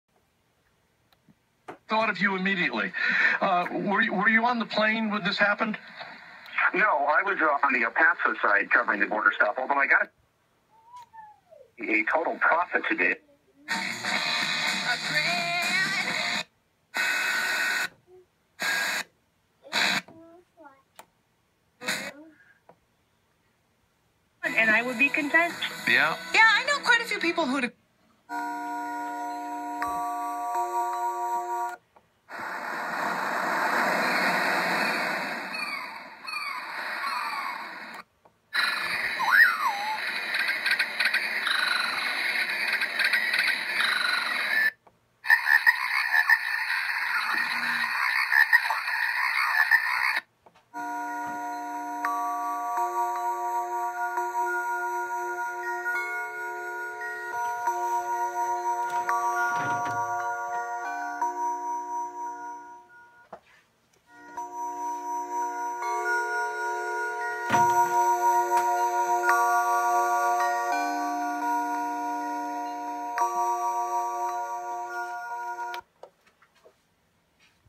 ex Dual Alarm Clock Radio Nature Sounds Noise Machine T3075 AM/FM MP3 Jack
Timex alarm radio with nature sounds

• nice pre-owned condition!

• The lettering for the sleep button is worn

• Has several different nature sounds, am/fm, and in audio in jack for MP3 player

• Features two alarms